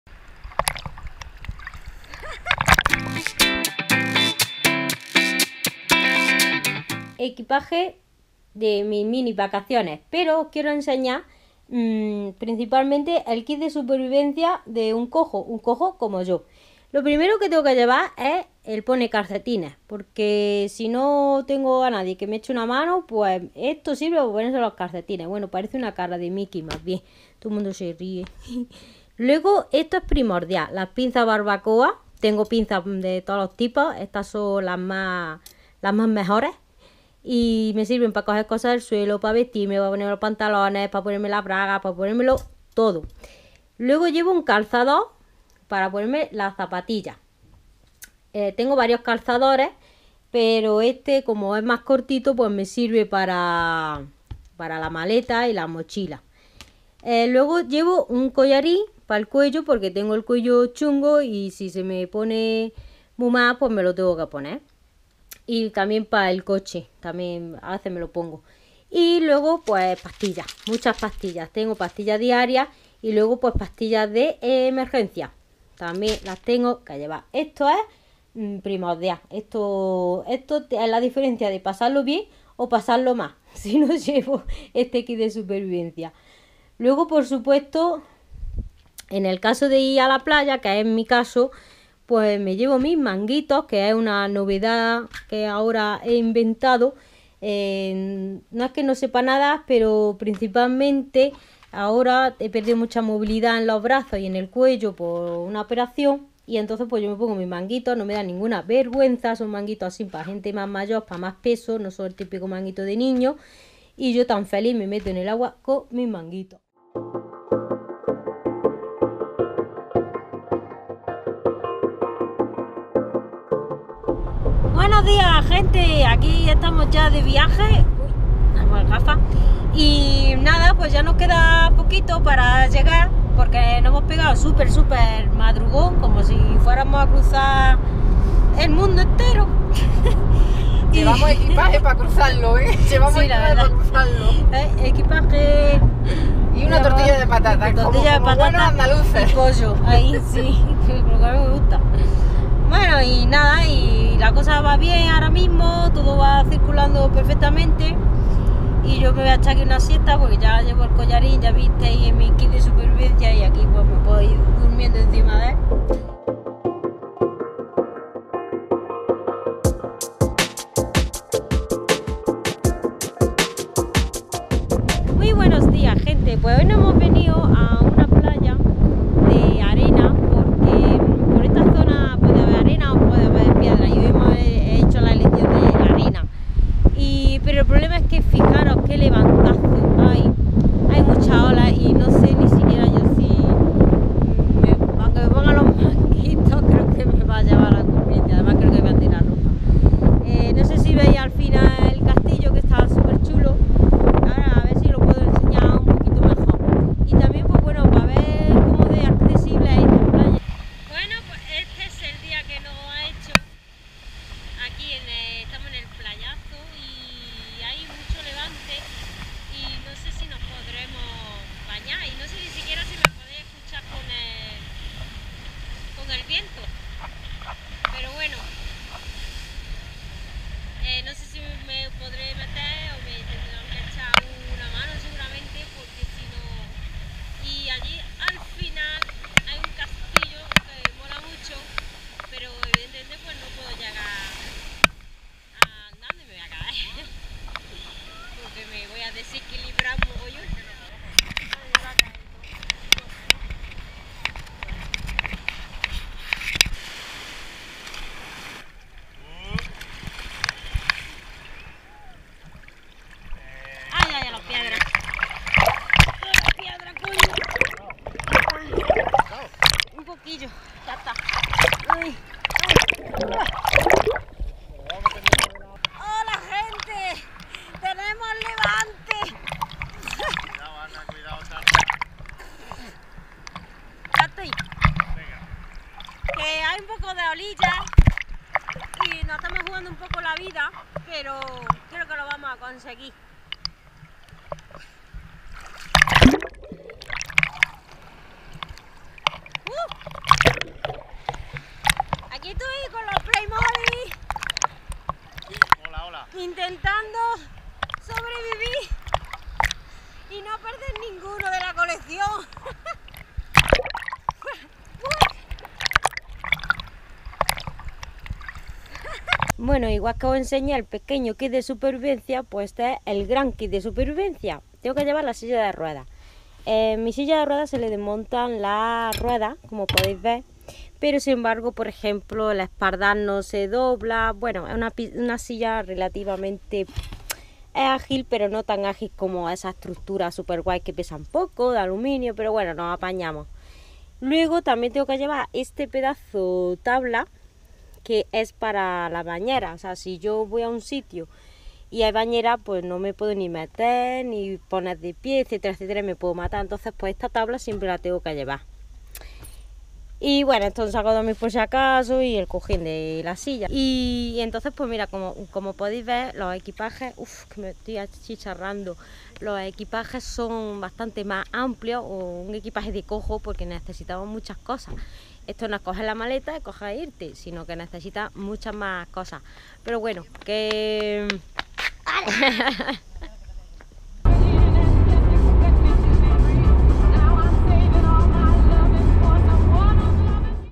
¡Una COJA en la playa!
Vlog "una coja en la playa", sobre mis minis vacaciones en Cabo de Gata, Almería.

¿Os gustan los Playmobil? en mi canal Rayada Play, encontrareis unboxing, información, exposiciones...👇